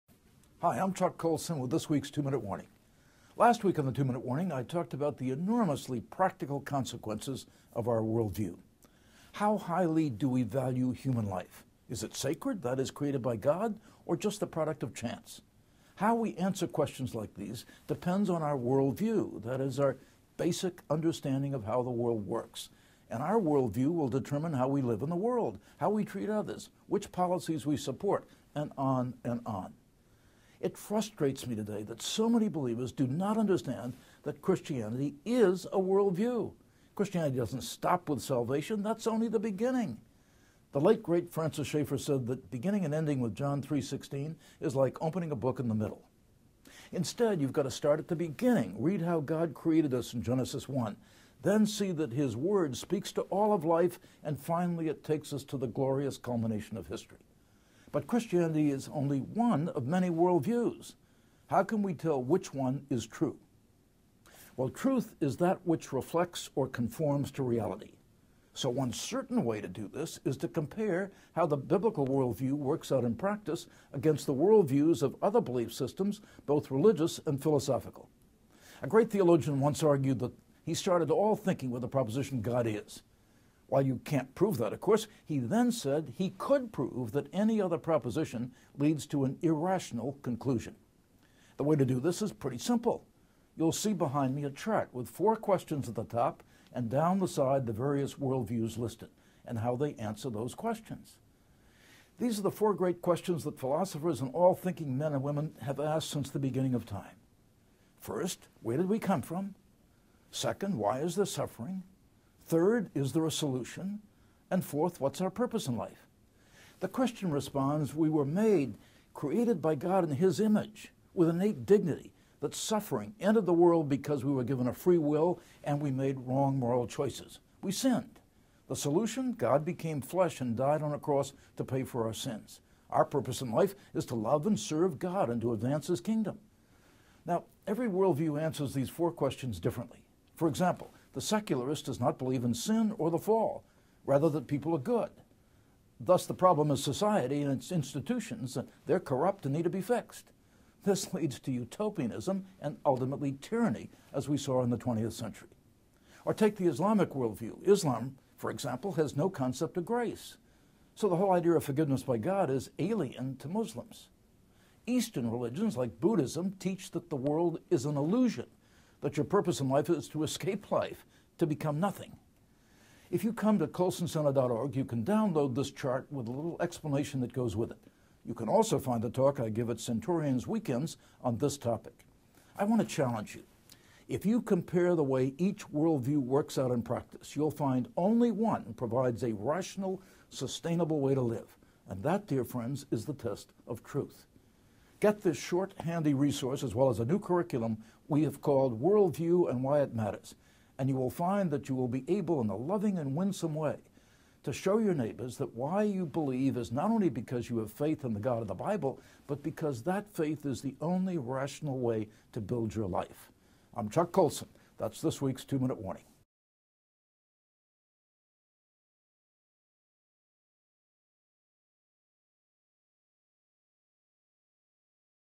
Two-Minute Warning: Answering Four Questions
Churches are not teaching their members how to think Christianly. While salvation is the difference between everlasting life and death, it is only the beginning of the Christian walk. This week, find out how to compare differing belief systems with a biblical worldview.